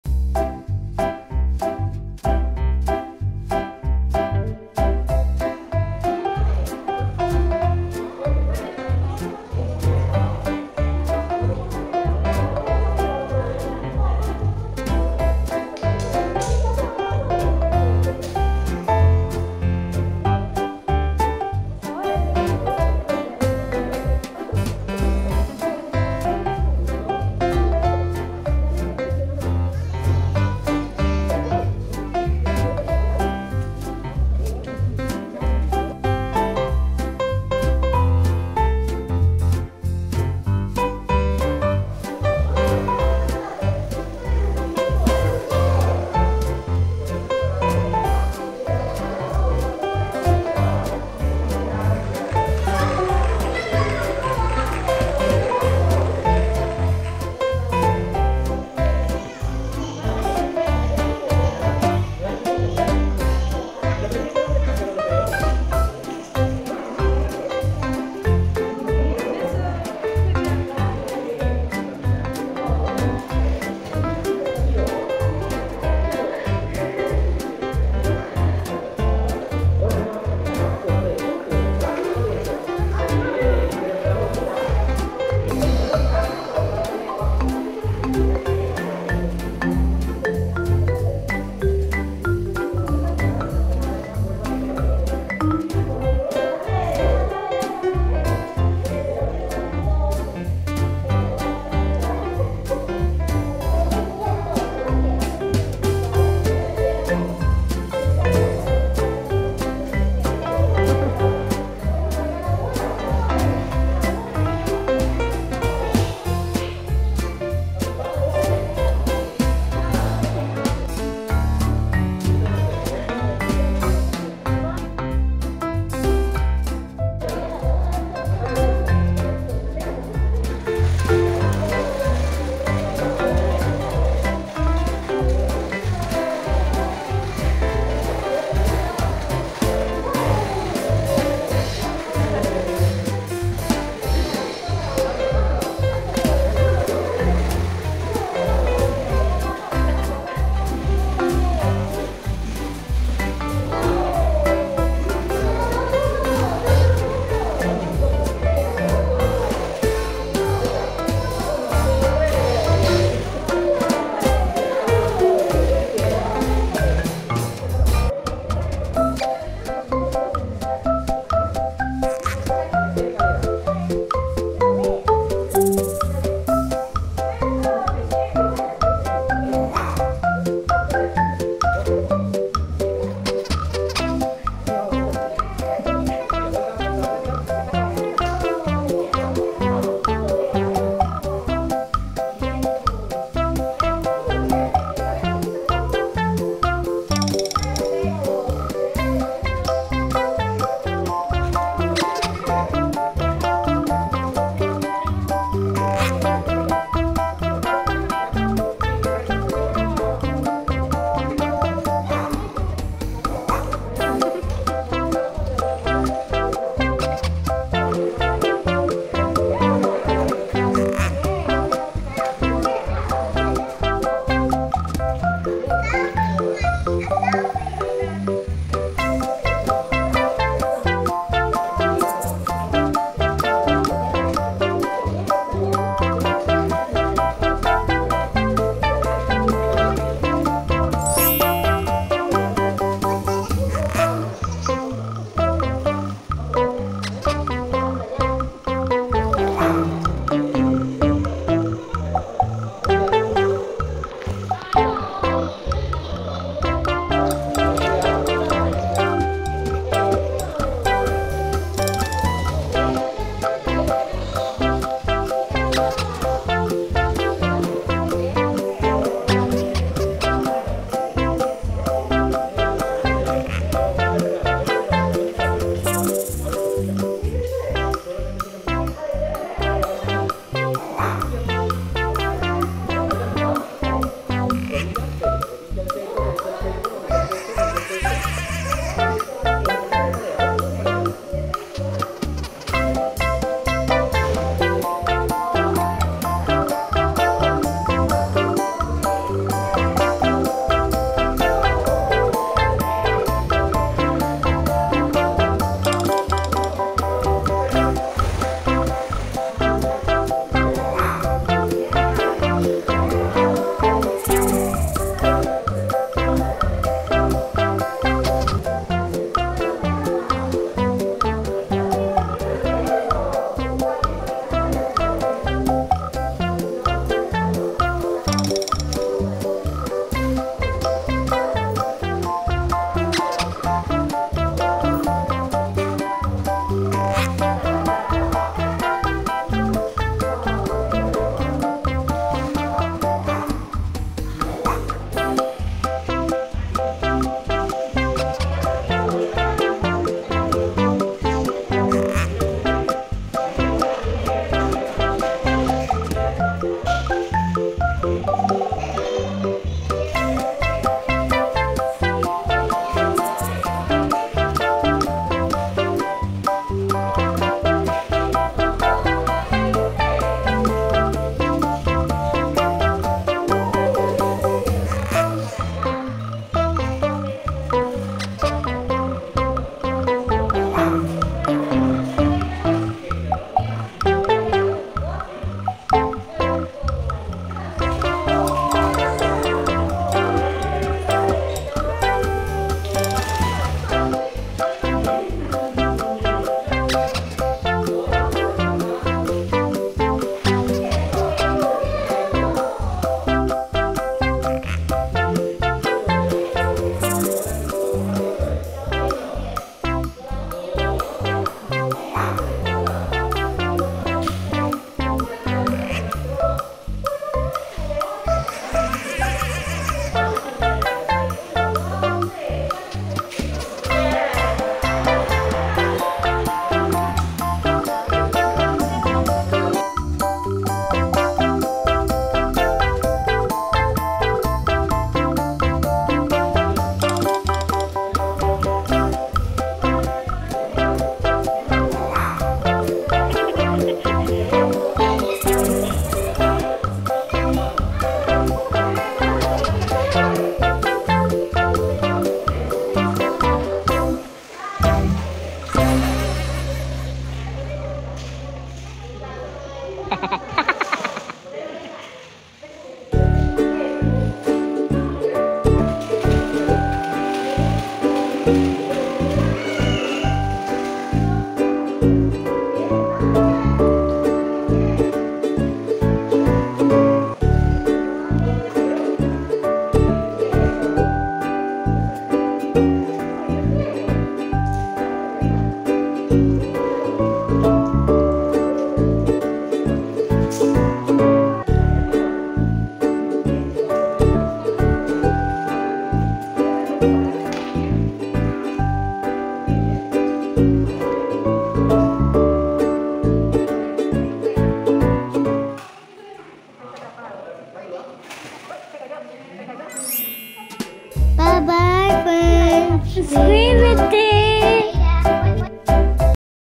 Soe y Alonso aprenden a nadar..📢🤔Quién lo hará primero❓😁swimming classes, alberca🏊piscina.
saludos a todos nuestros familiares y amig@s de YouTube 🥰
🤗un recuerdo más de nuestras actividades 👍🥰ellos están aprendiendo juntos a estos dos maestros súper que geniales...